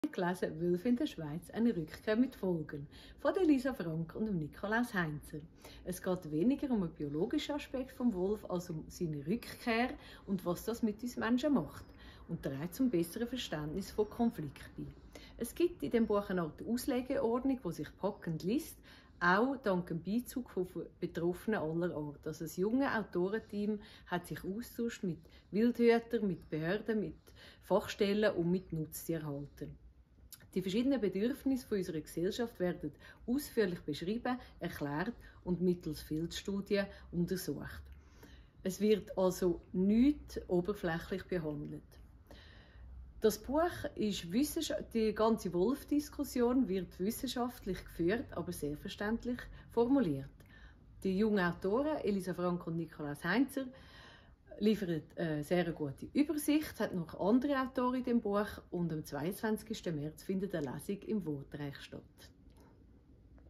Wölfe in der Schweiz» Eine Rückkehr mit Folgen
Lesung im Wortreich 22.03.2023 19:30 Uhr
1 Woche 1 Buch. Die Woche hani gläse
«Wölfe in der Schweiz» Eine Rückkehr mit Folgen  von Elisa Frank und Nikolaus Heinzer enthält Beiträge von Bernhard Tschofen, Lukas Denzler und Gianna Molinari
HIER UND JETZT VERLAG
Mehr: